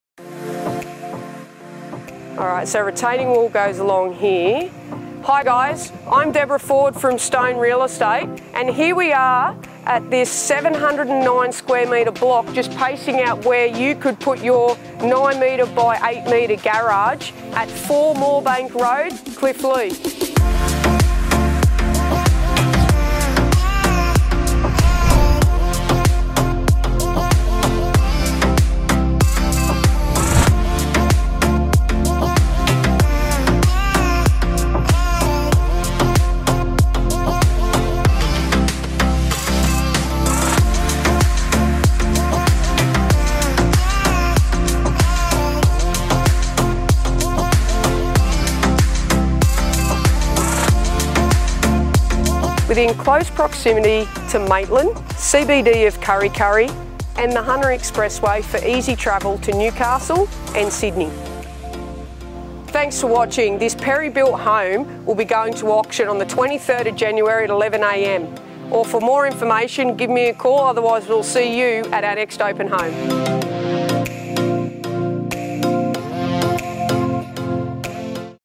A family home of comfort and style
🔥Just Listed🔥

🏡4 Moorebank Road, Cliftleigh

4 🛌 2🛀 2🚙

💰 Auction

⏰ Open homes every Saturday 19/12 & 16/1 @ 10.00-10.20am, 23/1 @ 10-10.30am

🕰 Auction on site 23/1 @ 11.00am

This expansive family home boasts a perfectly flowing layout of spacious living zones. Built by Perry Homes in 2017, this home enjoys perfectly presented interiors across a single level layout. It offers side access with room to build a shed or park the caravan or boat. Easily accessible to Maitland, Kurri Kurri CBD and the Hunter Express which makes travelling to Newcastle and Sydney a breeze.
- Perfectly designed family home, quality built from top-to-bottom
- Side access with room to build a shed to the side of the block
- Open plan living, dining and kitchen zone with alfresco access
- Flawless kitchen showcasing stone benchtops and premium appliances
- Separate formal lounge or media room, well-scaled for comfort
- Four generous bedrooms, master with 'must have' ensuite and WIR
- Built-ins to all other bedrooms, full main bathroom, well-equipped laundry
- Ducted air-conditioning, auto double garage with internal access
- Covered alfresco covered area invites an evening drink or outdoor dining
- 7.8 kw solar system and gas hot water system
- 5.4km from Kurri Kurri and only 11km to Maitland's CBD
- Ready to enjoy with no more to spend, an ideal family opportunity

To find out more information click on the link below 🔽🔽🔽🔽🔽🔽🔽🔽🔽🔽🔽🔽🔽🔽🔽🔽🔽